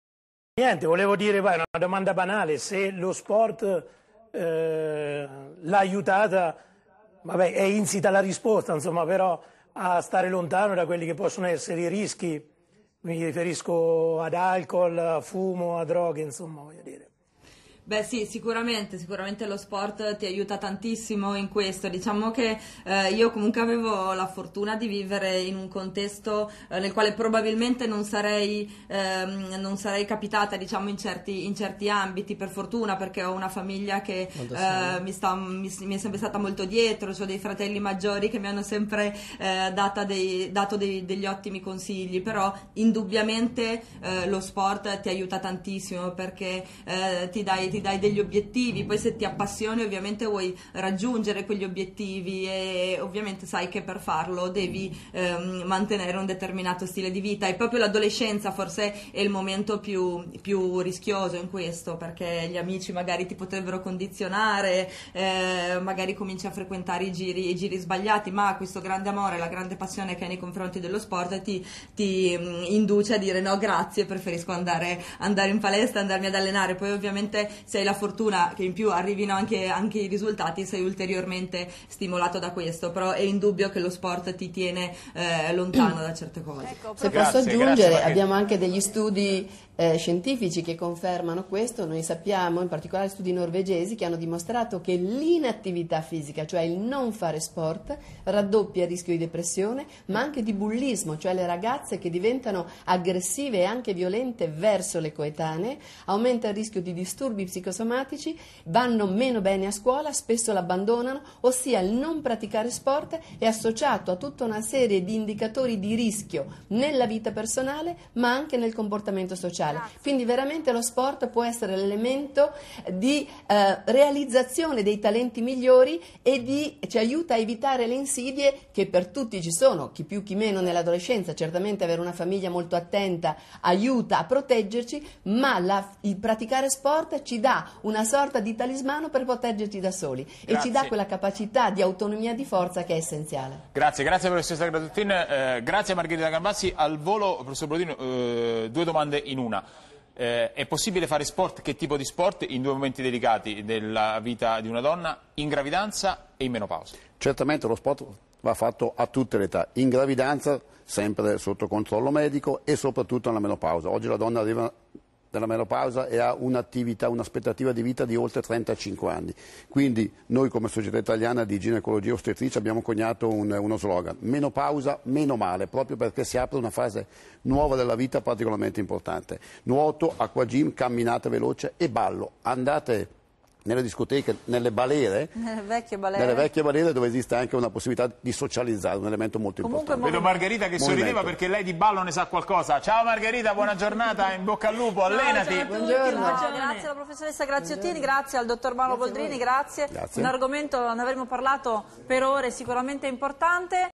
Sport e giovanissime: una priorità medica ed educativa - Parte 3
Lo sport protegge le adolescenti dai rischi connessi al fumo, al consumo di alcol e alla dipendenza da droghe. Soprattutto quando alle spalle c'è anche una famiglia solida e attenta. Ma secondo recenti studi scientifici, l'attività fisica è correlata anche ad altri importanti indicatori di vita personale e comportamento sociale. Leggi quali.